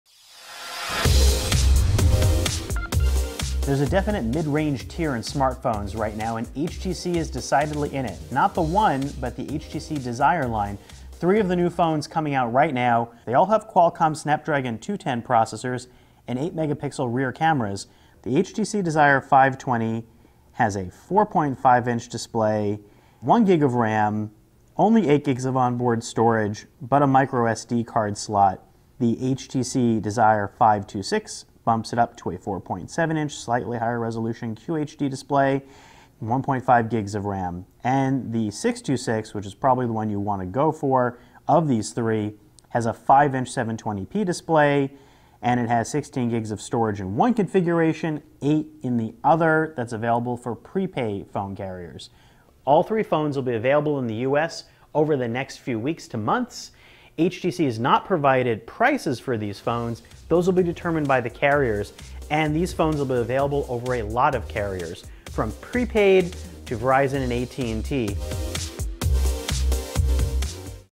HTC goes across carriers with Desire 520, 526, 626 and 626S
Mid-tier features and few surprises, but the new HTC Desire phones could at least be affordable, all have LTE and run Android Lollipop.